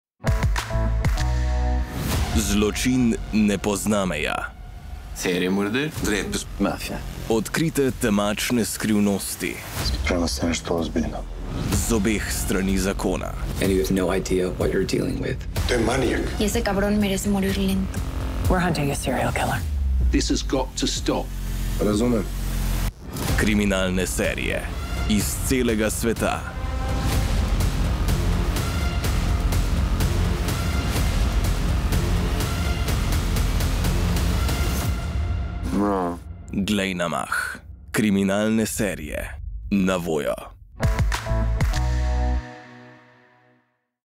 VOYO | Kriminalne serije | Si za binganje kriminalnih serij na VOYO?
🔍 Ste pripravljeni na vznemirljivo popotovanje skozi najtemnejše kotičke kriminalnega žanra?

🔍 Na VOYO vam predstavljamo kriminalne serije, ki vas bodo popeljale v svet skrivnosti, prevar in nepredvidljivih preobratov! 💥 Od raziskovanja zapletenih primerov do lovov na hude zločince - naša kriminalna kolekcija ima vse! Pridružite se nam 🔥

Sledi nam še na ostalih družbenih omrežjih in ne zamudi ekskluzivnih vsebin! 😎

Če še nisi, aktiviraj VOYO račun in glej vse vsebine že danes, kjerkoli in kadarkoli!